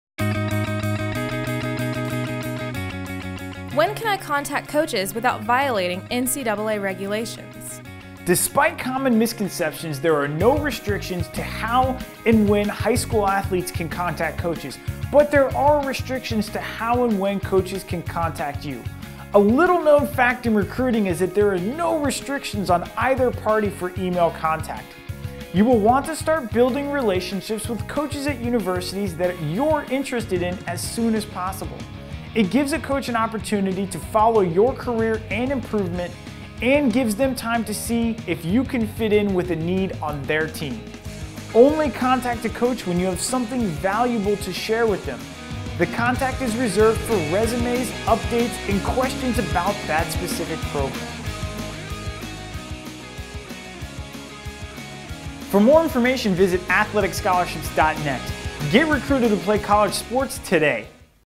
How to contact coaches without violating NCAA regulations
Contacting coaches before your We scoured the NCAA, NAIA, & NJCAA rule books to come up with a comprehensive list of when you can expect coaches to be calling you. Please keep in mind that just because they cannot contact you doesn't mean you cannot contact them. Use a medium like email to make your first initial contact and then always follow that email up with a friendly phone call introduction. If the coach doesn't answer, leave a nice message with a short introduction of who you are and why you were calling. Never say you were calling to talk about a scholarship, this is a very bad practice, instead that you were calling to introduce yourself, follow-up on the resume you emailed over, and to learn more about their program and never have your mom, dad, or coach call on your behalf. If you're calling before your senior year in high school there is never a need to leave a phone number but make sure to state your email address in case the coach wants to find the original submitted email.